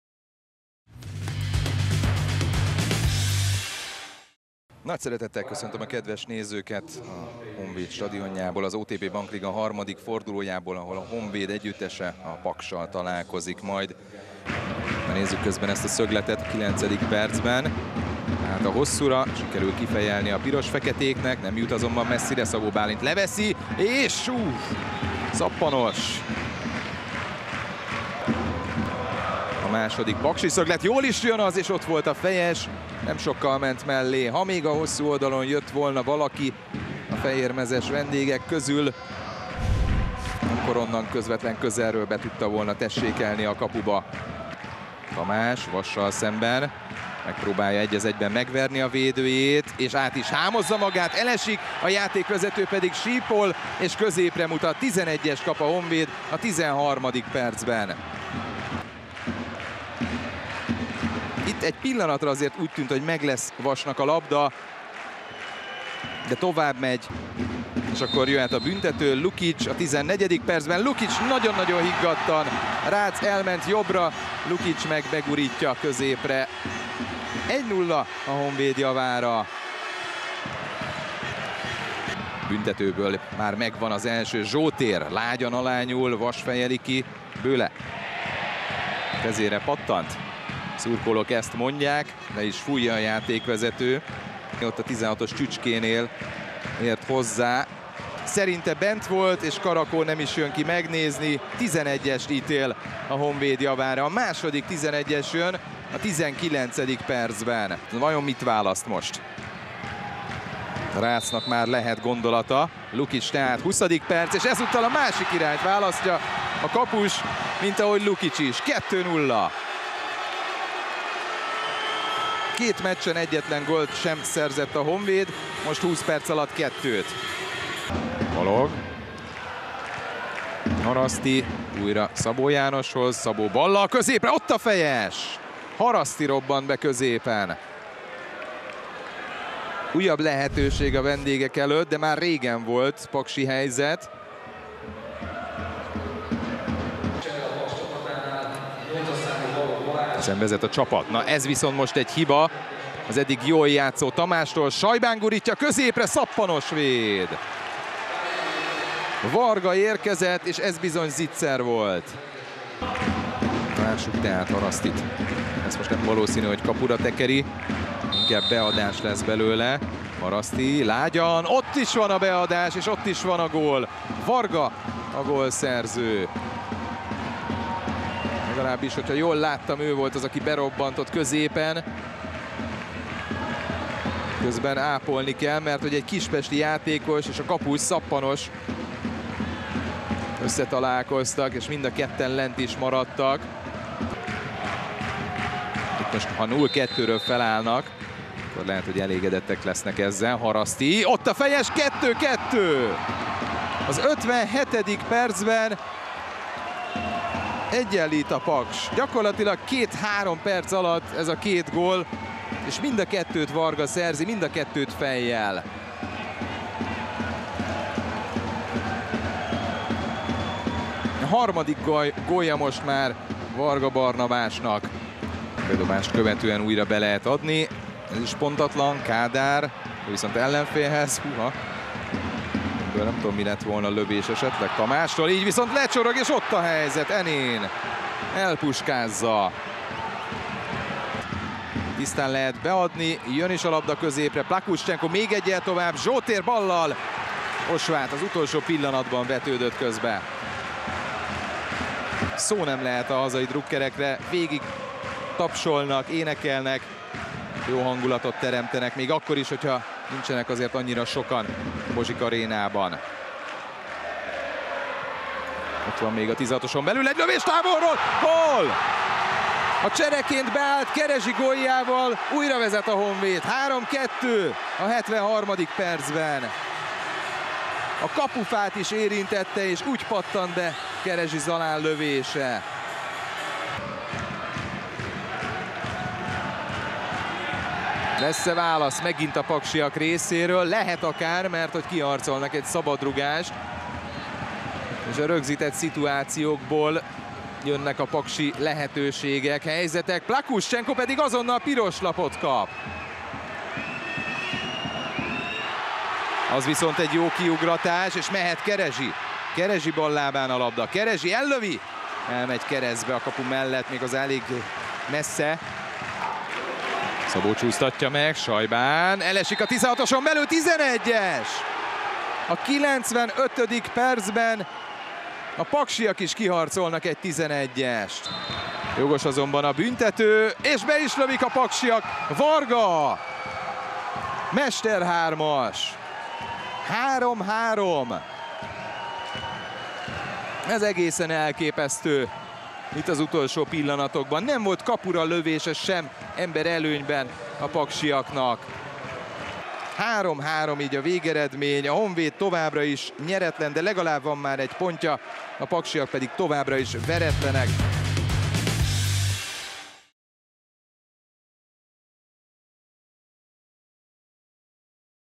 Budapest Honvéd - Paksi FC | 3-3 | (2-0) | OTP Bank Liga | 3. forduló | MLSZTV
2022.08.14. OTP Bank Liga, 3. forduló
Budapest Honvéd - Paksi FC | 3-3 | (2-0) |
összefoglaló